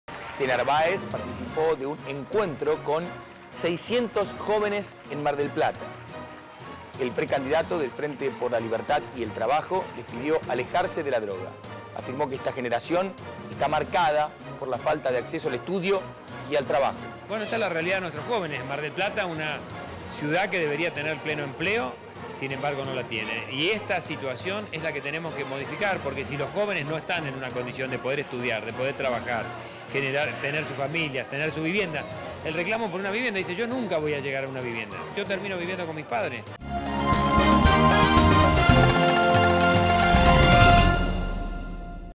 De Narváez en Mar del Plata - Telefe Noticias
El precandidato del "Frente por la libertad y el trabajo" se reunió con jóvenes en la ciudad balnearia y allí les pidió "alejarse de la droga".